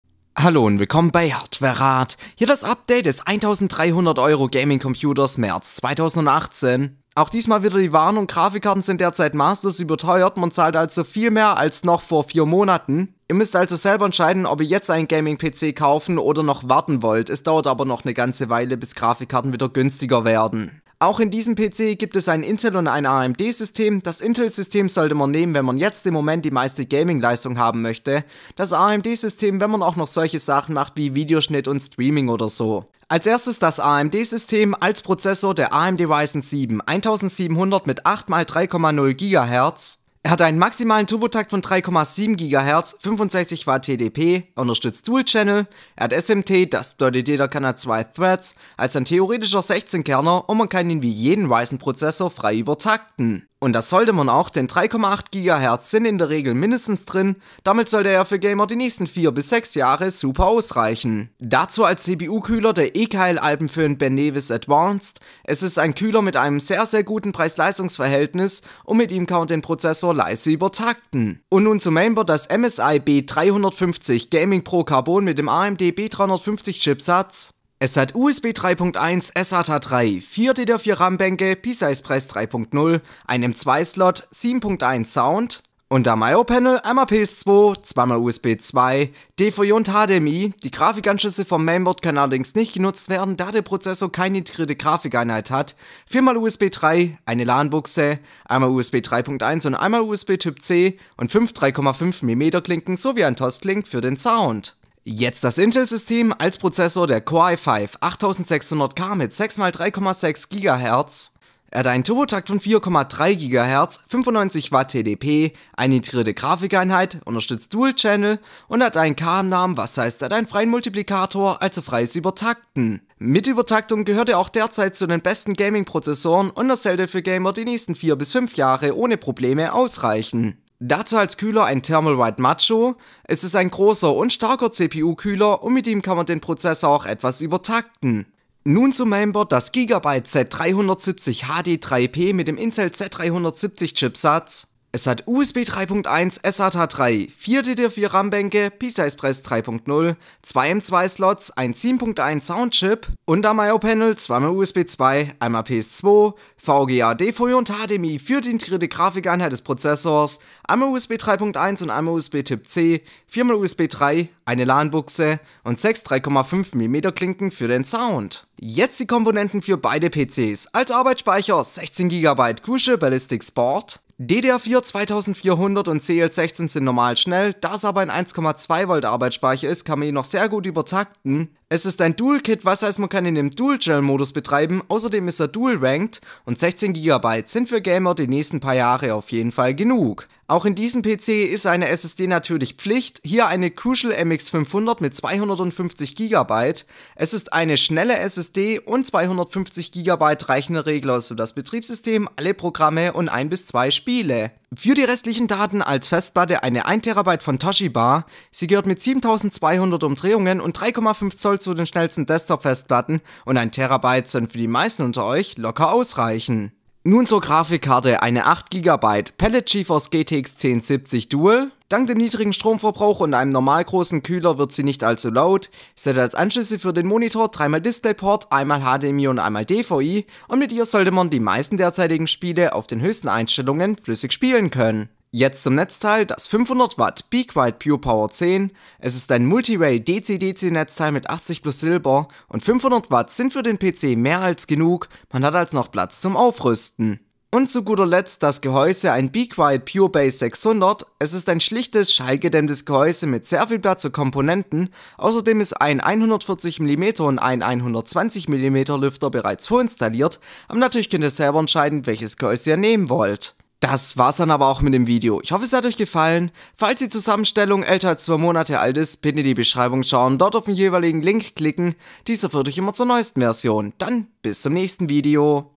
1300€ Gaming PC März 2018 | Intel & Ryzen + GTX 1070 | Computer günstig kaufen
In diesem Video seht ihr eine sehr gute PC Zusammenstellung in dem jeweiligen Preisbereich die für Gaming ausgelegt ist, also Gamingleistung steht hier ganz klar im Vordergrund.

(bei ebay günstiger, aber manchmal unseriöse volumenkeys)

Optional DVD Brenner